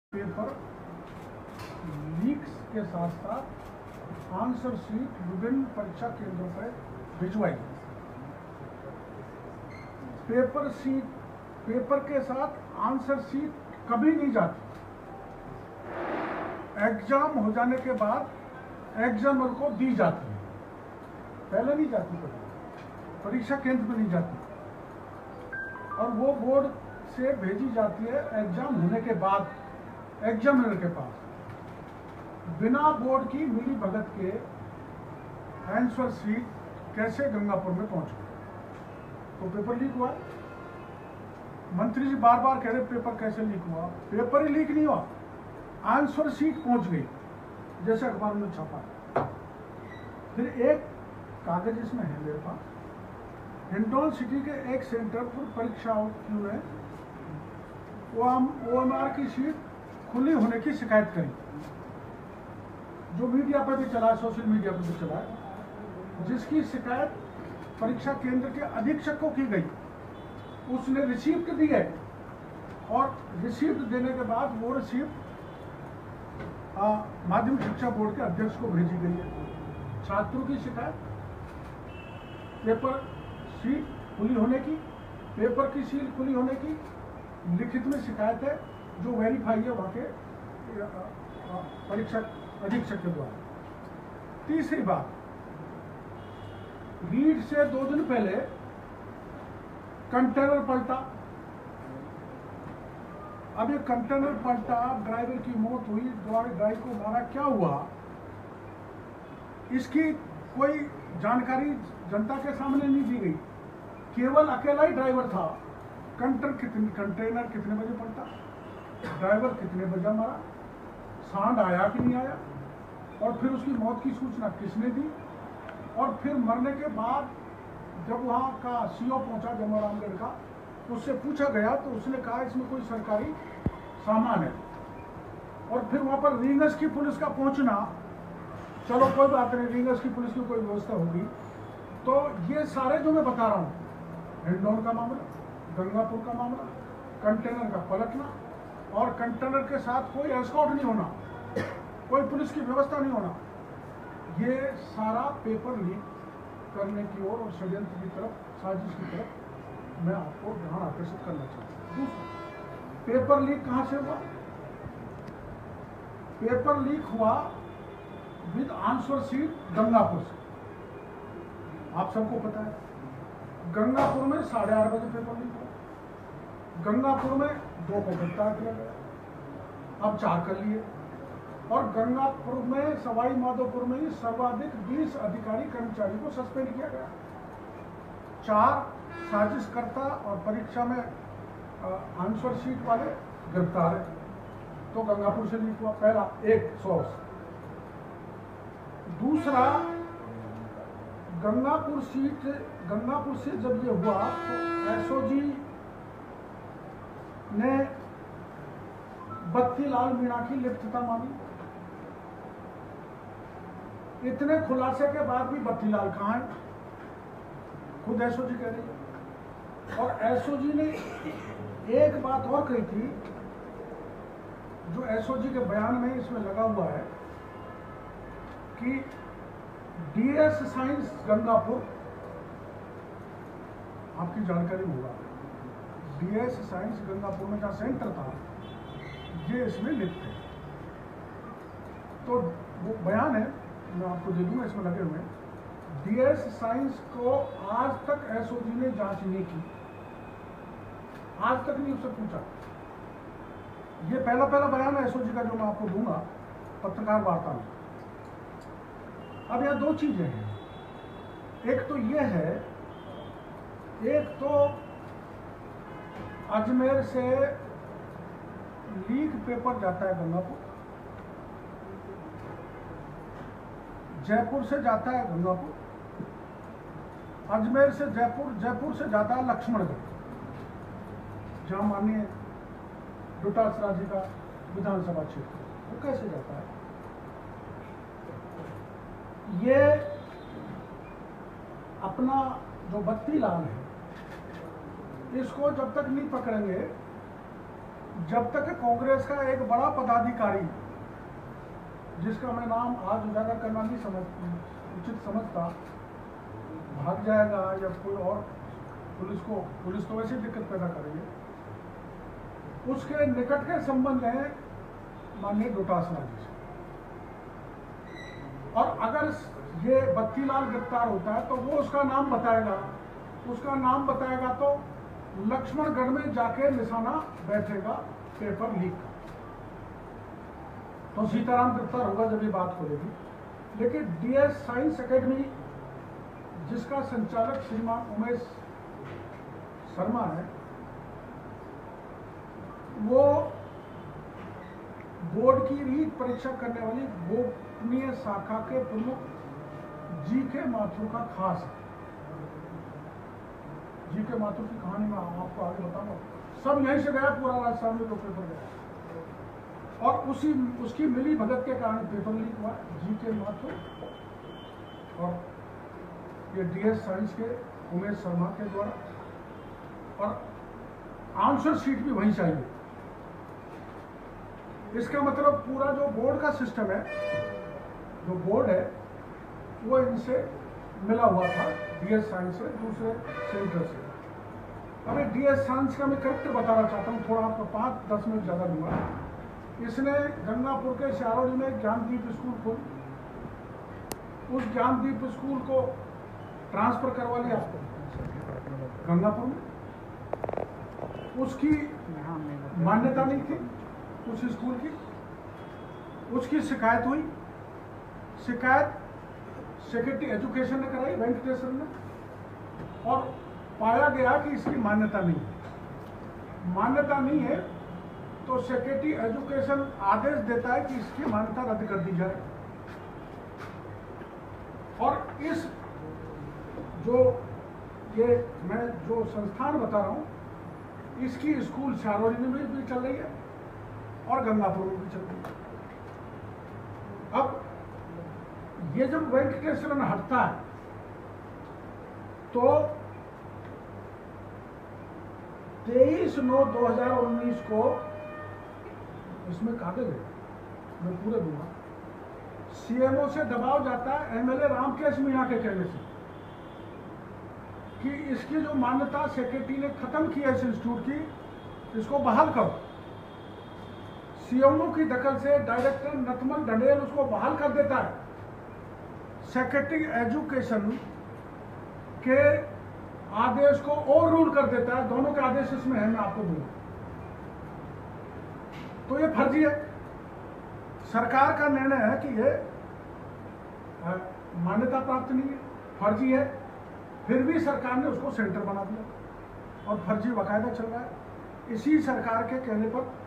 बाबा किरोड़ी जी की बड़ी press conference paper leak ke सबूत दिखाए कुछ दिन के लिए धरना स्थगित...
reet je si बाबा किरोड़ी जी की बड़ी press conference paper leak के सबूत दिखाए